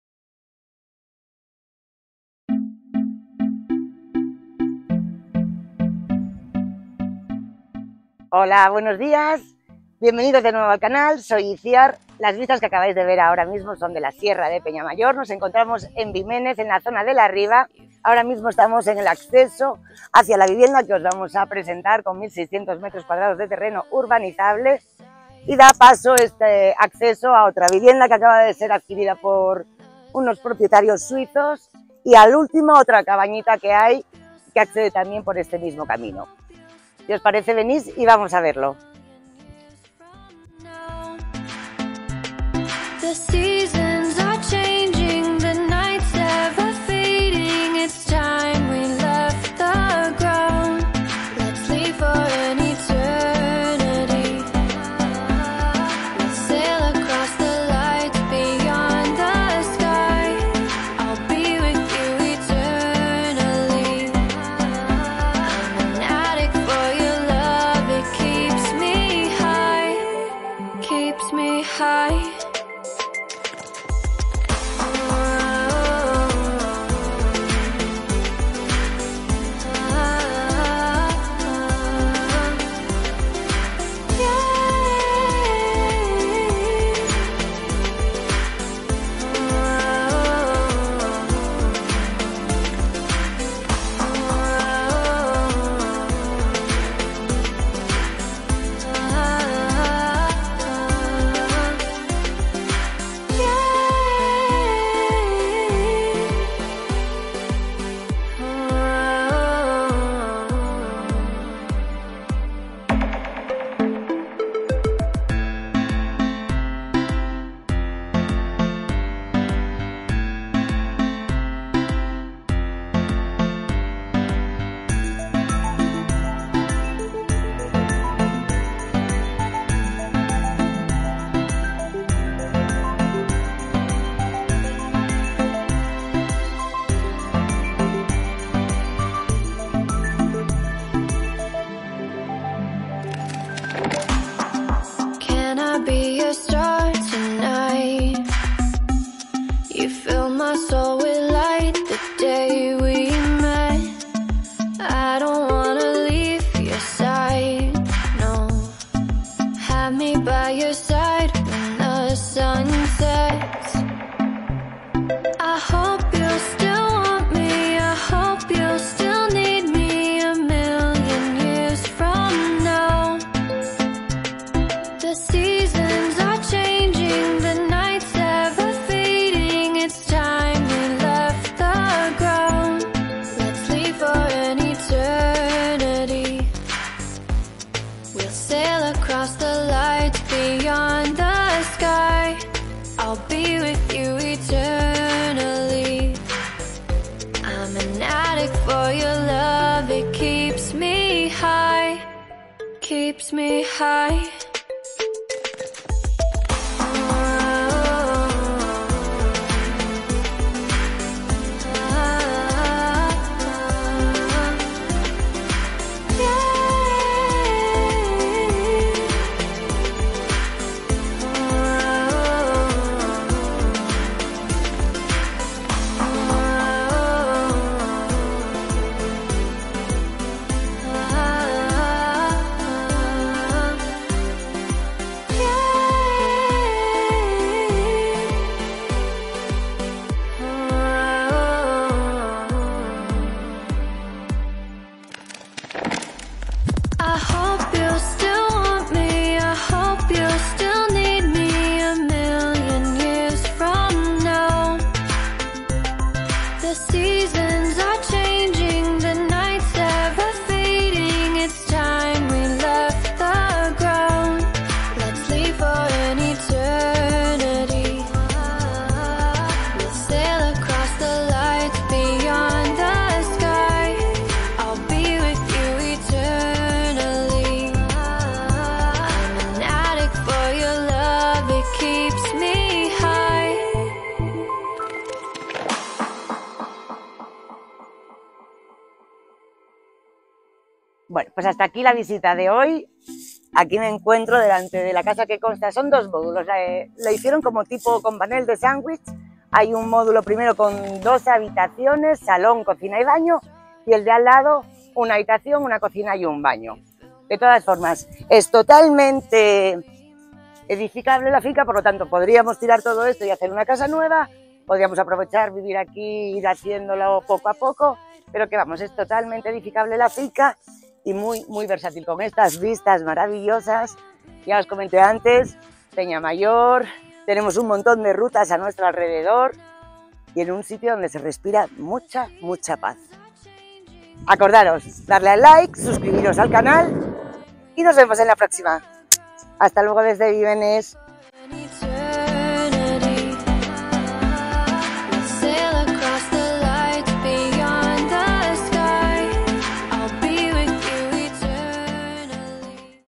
VENDIDO. A 55.000€ FINCA EN BIMENES, ASTURIAS CON TERRENO DE 1600M2
CASA EN VENTA EN CONCEJO DE BIMENES, ASTURIAS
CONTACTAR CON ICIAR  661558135
Se vende casa prefabricada en el concejo de Bimenes, Asturias. En pleno centro de Asturias y a tan solo 30 minutos de las playas más cercanas.
Esta propiedad es ideal para pasar los fines de semana o vacaciones, aunque al ser un terreno edificable se puede construir una casa para vivir de continuo. Tanto Oviedo como Gijón está a 30 minutos aproximadamente.
son dos edificaciones de chapa que se distribuyen de la siguiente manera.
La primera edificación cuenta con 1 cocina, un baño, 1 salita y dos habitaciones. en la segunda tenemos otra cocina, un segundo baño y 1 habitación.

El terreno es de 1600m2 y como decíamos en la descripción es edificable.
Vistas impresionantes a Peñamayor.

Para más información, podéis contactarnos, atendemos WhatsApp!!